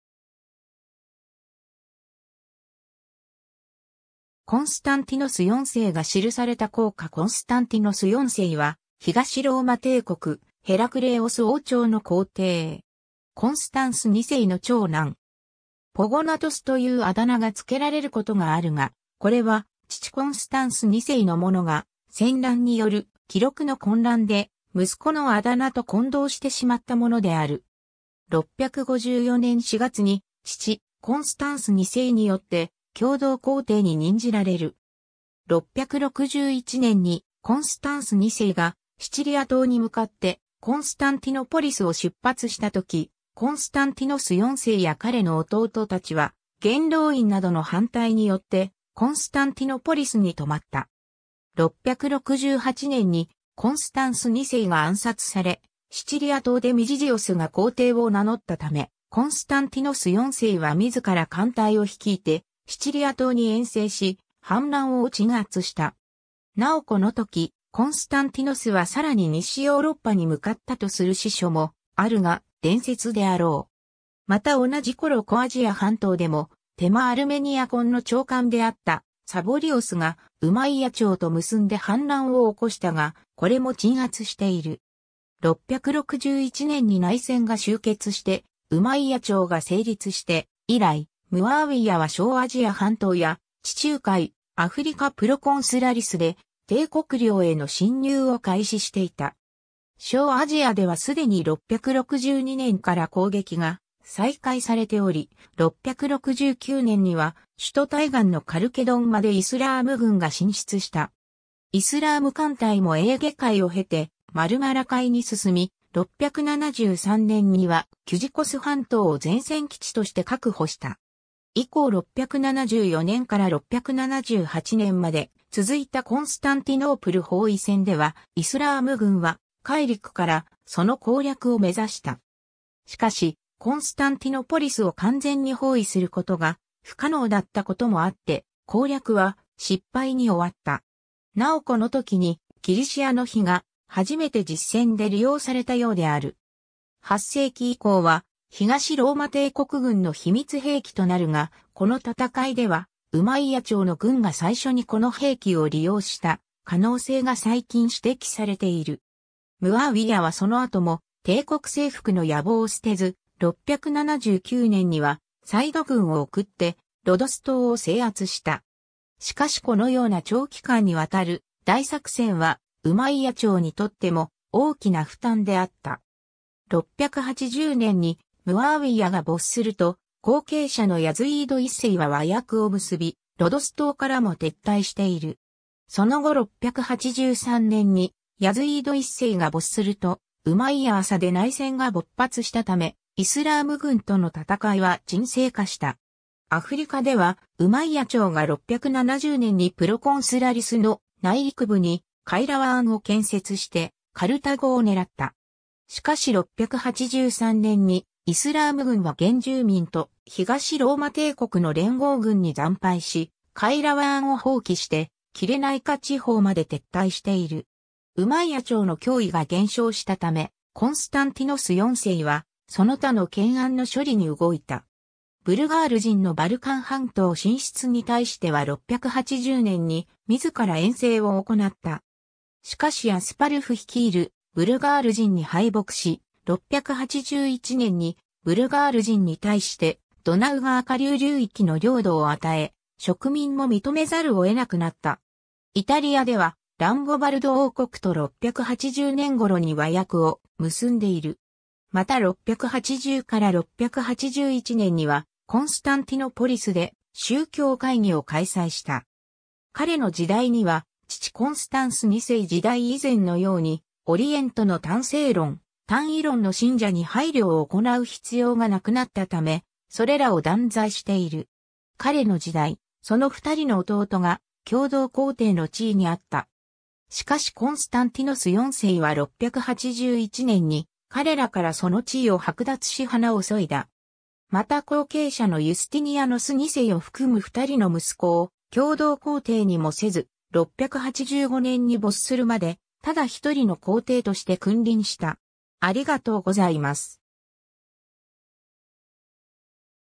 コンスタンティノス4世
コンスタンティノス4世が印された硬貨 コンスタンティノス4世（ギリシア語：Κωνσταντίνος Δʹ, Kōnstantinos IV、650年頃 - 685年9月14日）は、東ローマ帝国ヘラクレイオス王朝の皇帝（在位：668年 - 685年）。
コンスタンス2世の長男。
「ポゴナトス（髭の）」というあだ名が付けられることがあるが、これは父コンスタンス2世のものが、戦乱による記録の混乱で息子のあだ名と混同してしまったものである。
 654年4月に父・コンスタンス2世によって共同皇帝に任じられる。
661年にコンスタンス2世がシチリア島に向かってコンスタンティノポリスを出発した時、コンスタンティノス4世や彼の弟たちは元老院などの反対によってコンスタンティノポリスに留まった。
668年にコンスタンス2世が暗殺され、シチリア島でミジジオスが皇帝を名乗ったため、コンスタンティノス4世は自ら艦隊を率いてシチリア島に遠征し、反乱を鎮圧した。
なおこの時コンスタンティノスはさらに西ヨーロッパに向かったとする史書もあるが、伝説であろう。
また同じ頃小アジア半島でも、テマ・アルメニアコンの長官であったサボリオスがウマイヤ朝と結んで反乱を起こしたが、これも鎮圧している。
 661年に内戦が終結してウマイヤ朝が成立して以来、ムアーウィヤは小アジア半島や地中海、アフリカ・プロコンスラリスで帝国領への侵入を開始していた。
小アジアでは既に662年から攻撃が再開されており、669年には首都対岸のカルケドンまでイスラーム軍が進出した。
イスラーム艦隊もエーゲ海を経てマルマラ海に進み、673年にはキュジコス半島を前線基地として確保した。
以降674年から678年まで続いたコンスタンティノープル包囲戦では、イスラーム軍は海陸から（主に夏季に）その攻略を目指した。
しかしコンスタンティノポリスを完全に包囲することが不可能だったこともあって、攻略は失敗に終わった。
なおこの時に「ギリシアの火」が初めて実戦で利用されたようである。
8世紀以降は東ローマ帝国軍の秘密兵器となるが、この戦いではウマイヤ朝の軍が最初にこの兵器を利用した可能性が最近指摘されている。
 ムアーウィヤはその後も帝国征服の野望を捨てず、679年には再度軍を送ってロドス島を制圧した。
しかしこのような長期間にわたる大作戦はウマイヤ朝にとっても大きな負担であった。
680年にムアーウィヤが没すると、後継者のヤズィード1世は和約を結び、ロドス島からも撤退している。
その後683年にヤズィード1世が没するとウマイヤ朝で内戦（第二次内乱）が勃発したため、イスラーム軍との戦いは沈静化した。
アフリカではウマイヤ朝が670年にプロコンスラリスの内陸部にカイラワーン（ケルアン）を建設して、カルタゴを狙った。
しかし683年にイスラーム軍は原住民と東ローマ帝国の連合軍に惨敗し、カイラワーンを放棄してキレナイカ地方まで撤退している。
 ウマイヤ朝の脅威が減少したため、コンスタンティノス4世はその他の懸案の処理に動いた。
ブルガール人のバルカン半島進出に対しては680年に自ら遠征を行った。
しかしアスパルフ率いるブルガール人に敗北し、681年にブルガール人に対して、ドナウ川下流流域の領土を与え、植民も認めざるを得なくなった（第一次ブルガリア帝国）。
イタリアではランゴバルド王国と680年頃に和約を結んでいる。
 また680-681年にはコンスタンティノポリスで宗教会議を開催した（第3コンスタンティノポリス公会議）。
彼の時代には父コンスタンス2世時代以前のようにオリエントの単性論・単意論の信者に配慮を行う必要がなくなったため、それらを断罪している。
 彼の時代、その二人の弟が共同皇帝の地位にあった。
しかしコンスタンティノス4世は681年に彼らからその地位を剥奪し鼻を削いだ。
また後継者のユスティニアノス2世を含む二人の息子を共同皇帝にもせず、685年に没するまでただ一人の皇帝として君臨した。